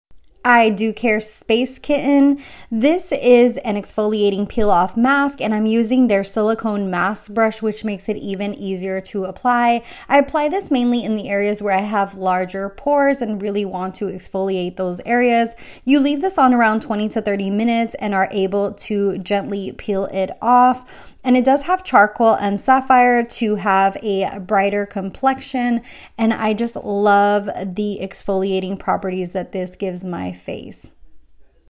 Charcoal Peel Off Mask
@idewcare Space Kitten 🐈‍⬛ Mask
$23

The I Dew Care Space Kitten Exfoliating Charcoal Peel-Off Mask helps exfoliate whiteheads and brighten the skin.

Silicone Face Mask Brush $10

*gifted for review